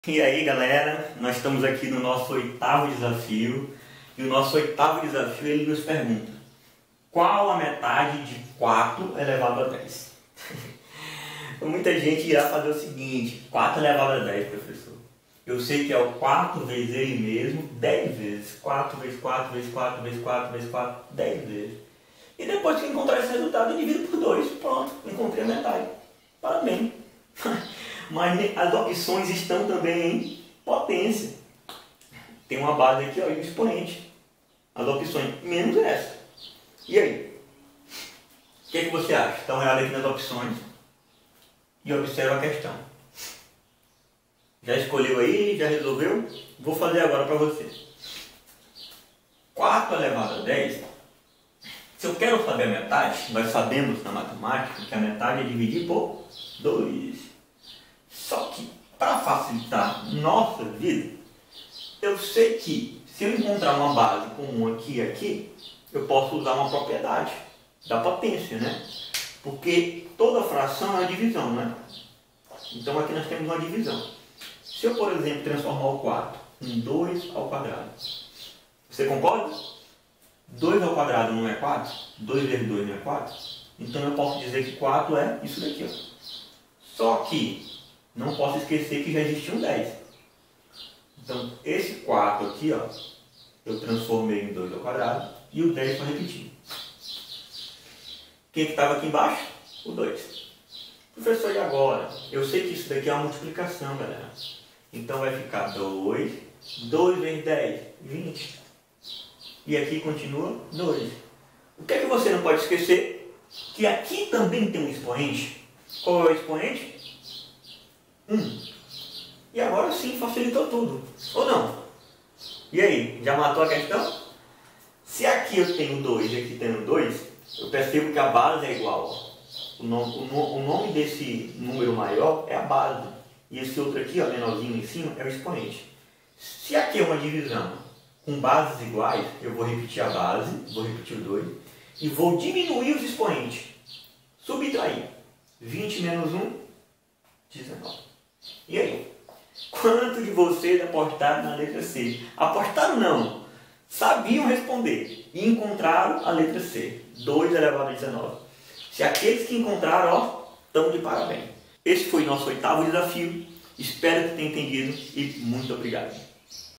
DESAFIO #08 (METADE DE UMA POTÊNCIA)
NESSE VÍDEO EXPLICO DE FORMA SIMPLIFICADA COMO DESCOBRIR A METADE DE UMA POTÊNCIA.

DESAFIO #08
QUAL A METADE DE 410 (quatro elevado a dez)?
A) 210 (dois elevado a dez)
B) 45 (quatro elevado a cinco)
C) 219 (dois elevado a dezenove)
D) 0

FACEBOOK: Hyago Willes Barros Lima